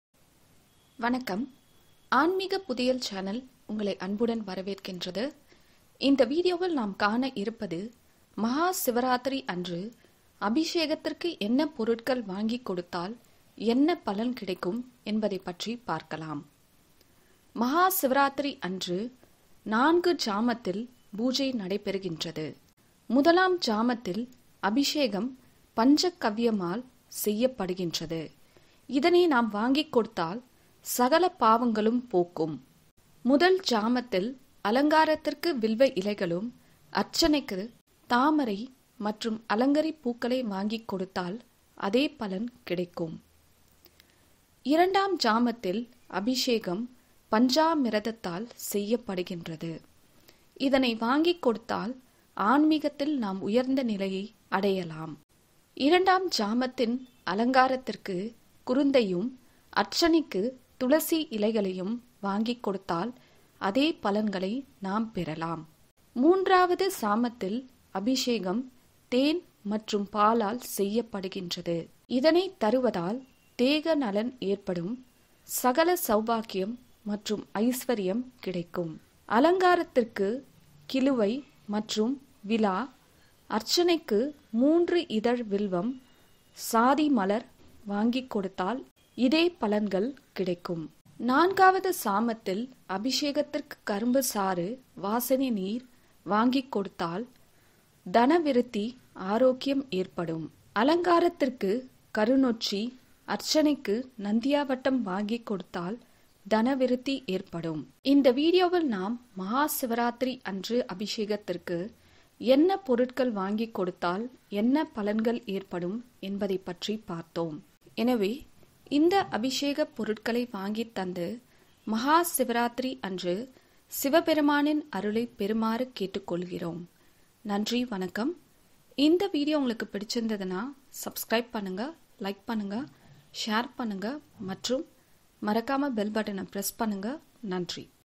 "மஹா சிவராத்திரி" அன்று அபிஷேகத்திற்கு எந்த பொருட்கள் வாங்கி தரவேண்டும் / பலன்கள்/ Maha Sivarathiri
இந்த வீடியோவில் நாம் "மஹா சிவராத்திரி" அன்று அபிஷேகத்திற்கு எந்த பொருட்கள் வாங்கி தரவேண்டும் என்பதை பற்றி தெரிந்து கொள்ளலாம்.